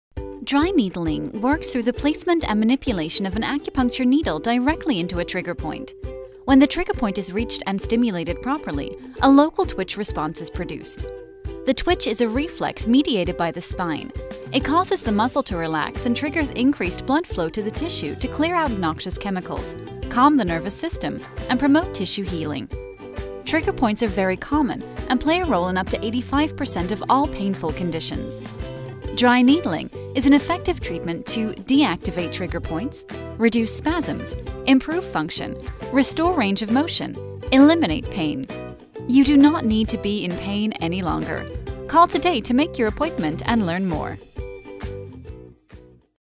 Dry Needling - Charlotte Acupuncture and Wellness Center
Brief description of dry needling and its positive effects. Produced by Charlotte Acupuncture and Wellness Center located in Charlotte, NC.